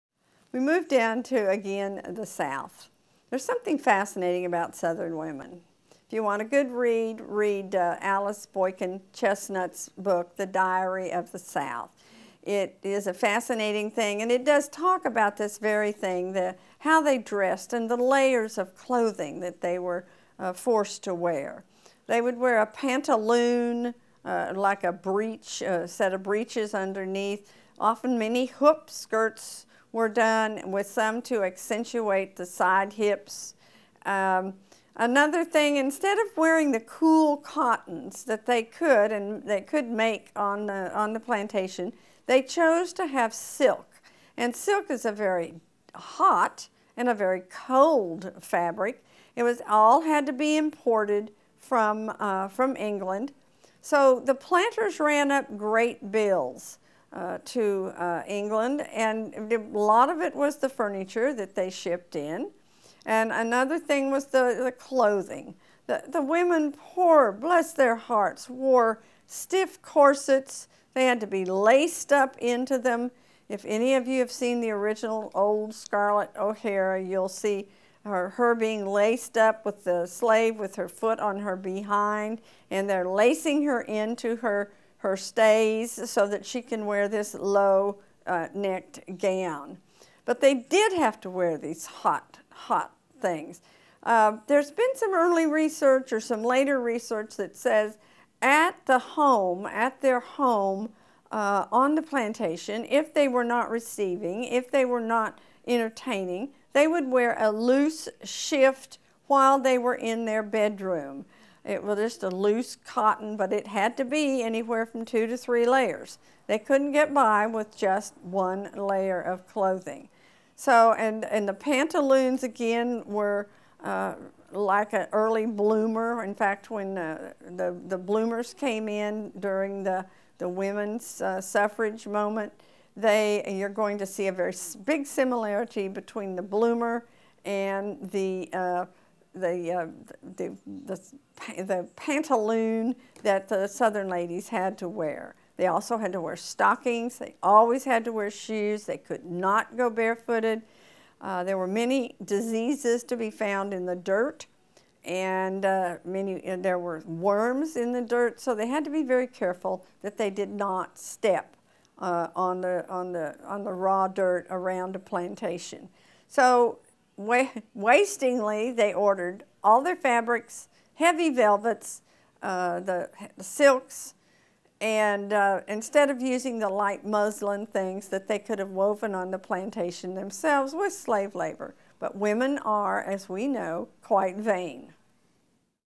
American History : About the Clothing of the Southern Colonies
The clothing of southern colonies is marked by pantaloons, layers of clothing, hoop skirts, stiff corsets and stockings. Find out why people were often wary of going barefoot in the southern colonies with help from an American history teacher in this free video on colonial life.

Expert: Judy Scott
Bio: Judy Scott is a retired, award-winning American history teacher in Ft. Worth, Texas. She was the AP history teacher at Boswell High School for three decades.
Filmmaker: Kevin Haberer